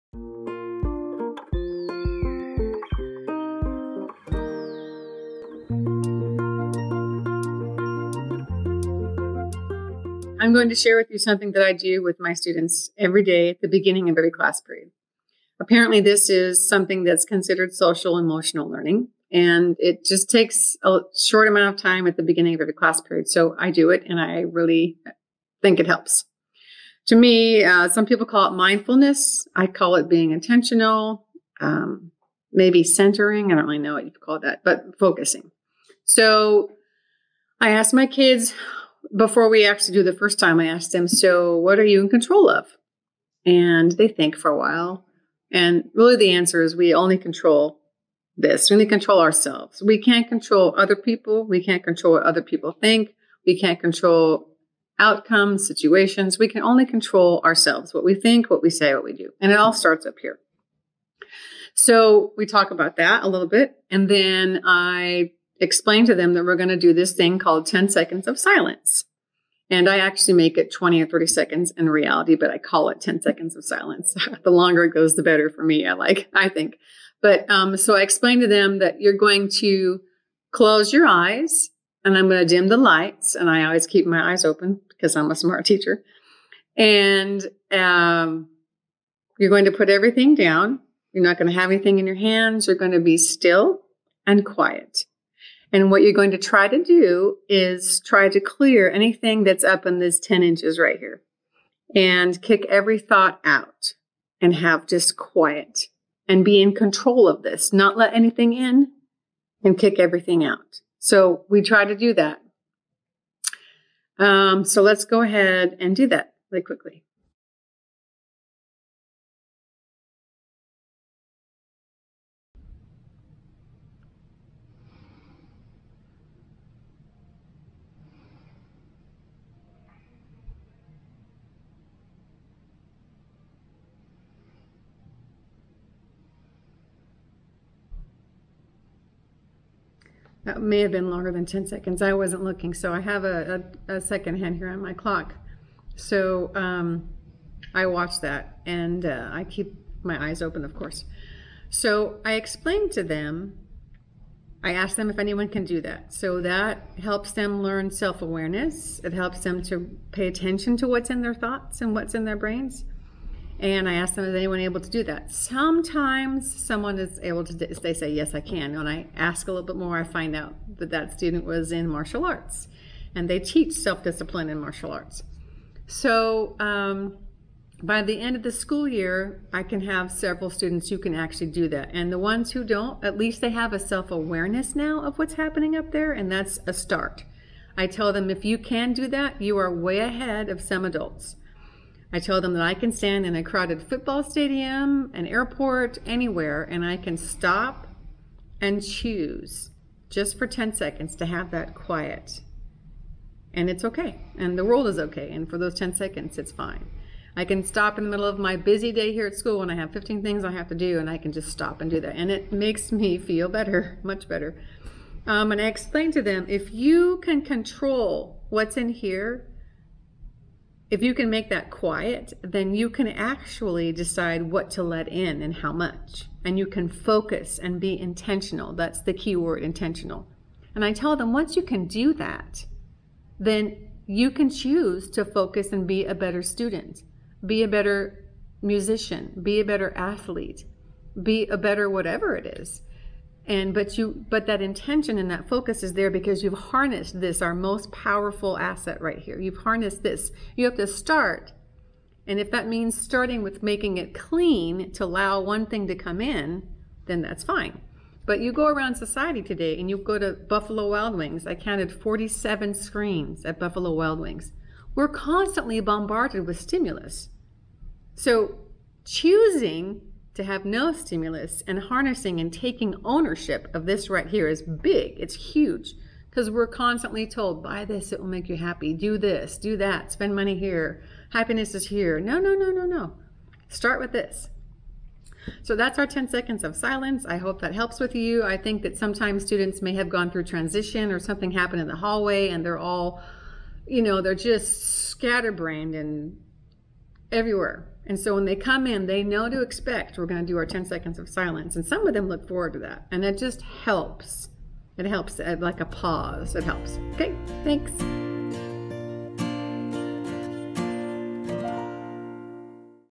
Beaumont ISD Teacher Explains SEL/Mindfulness in the Classroom - Beaumont United High School
Teacher from Beaumont United shares a great tip on how 10 seconds of silence can be used in the classroom to apply SEL and Mindfulness.